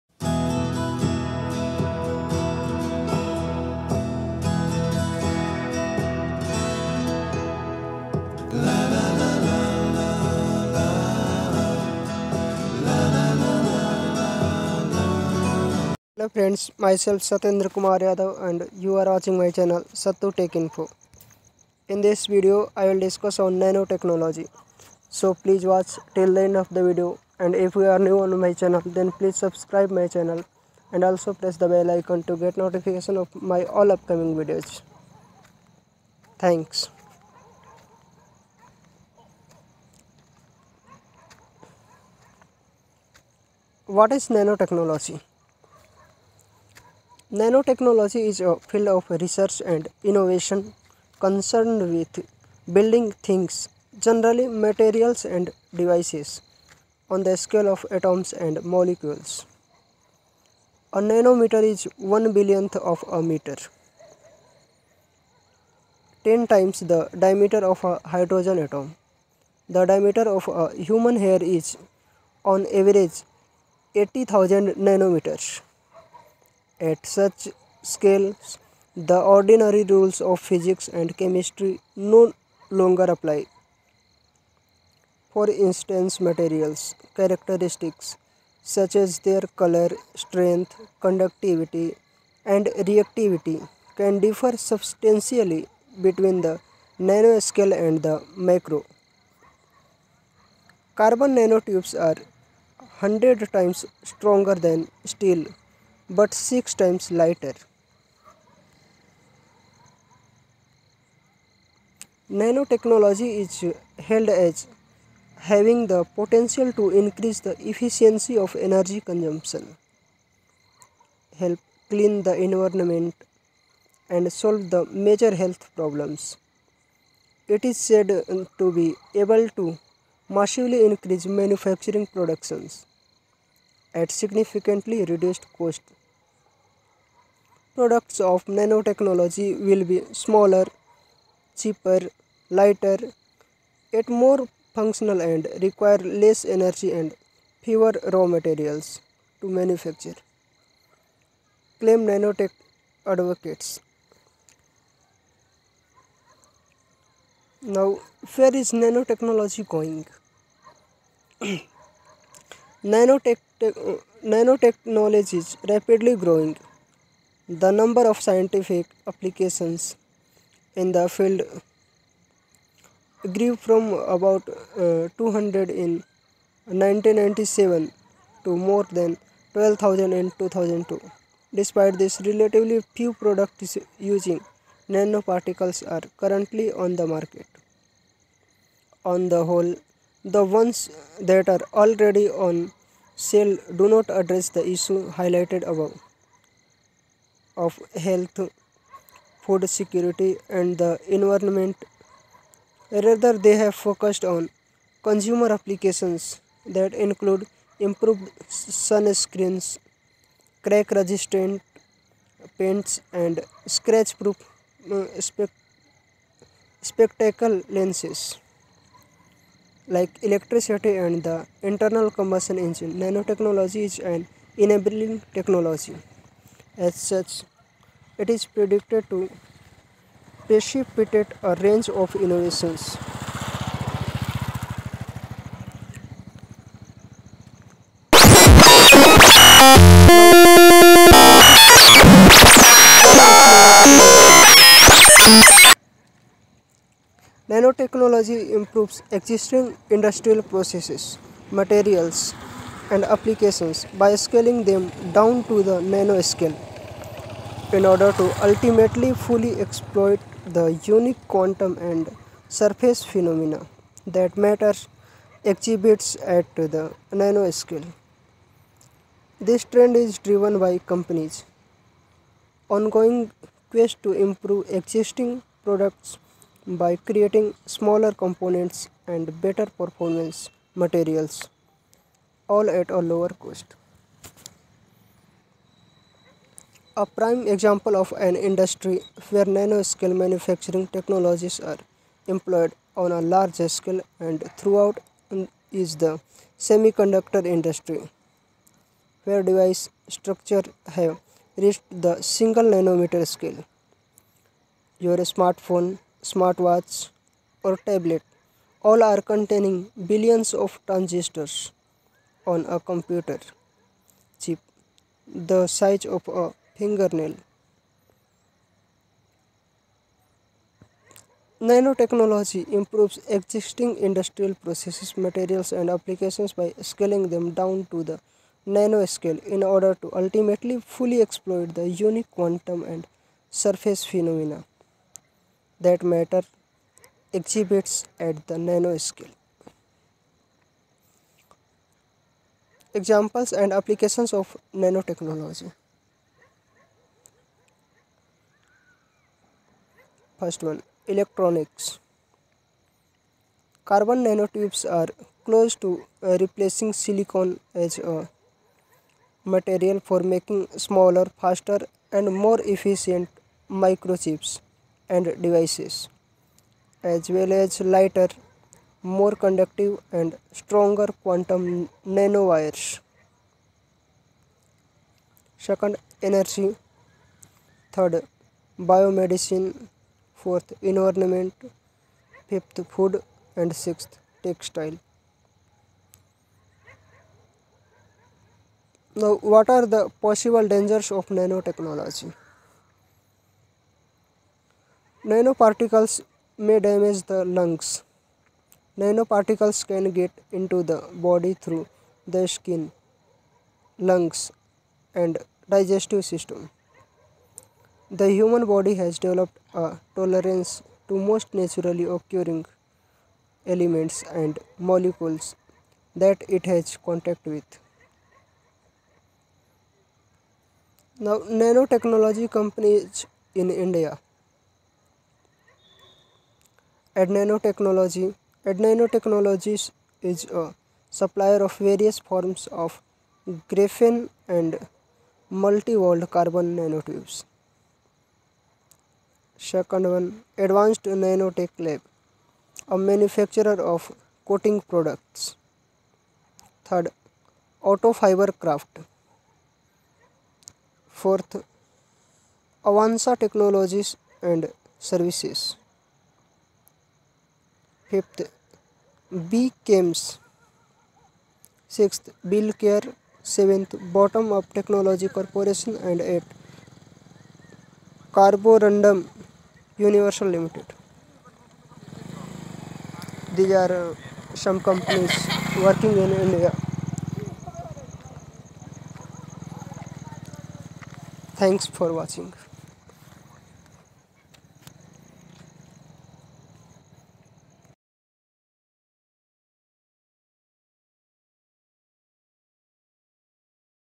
Nanotechnology / Nanoscience (NT / NS)
✓ What is Nanotechnology & Nanoscience ?
✓ Why nanotechnology is the futuristic technology ?
✓ Where Nanotechnology is going ?
✓ What are the applications of Nanotechnology ?
✓ How Nanotechnology can change the futuristic Technologies ?